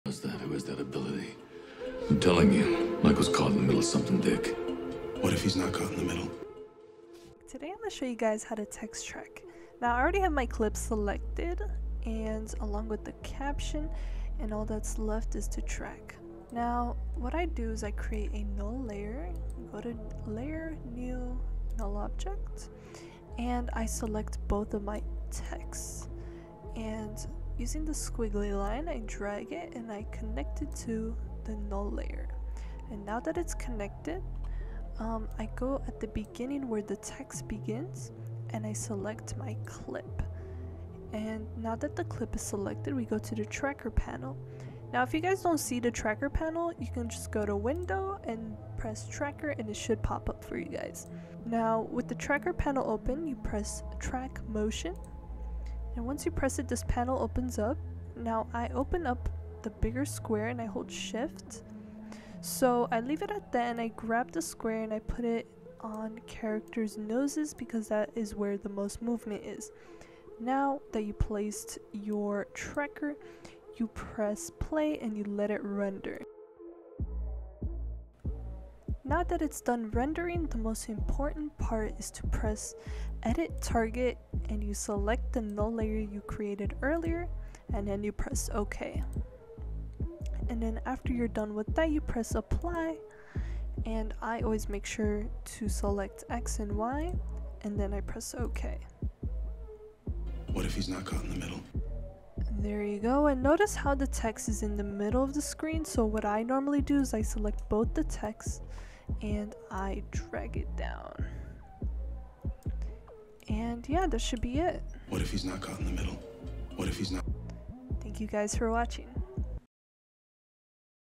How to text track in After Effects
👋 In this video, I will show you how to do Text Tracking in After Effects.

Sorry for the glitches in the video.
Comment what tutorial you guys want to see next.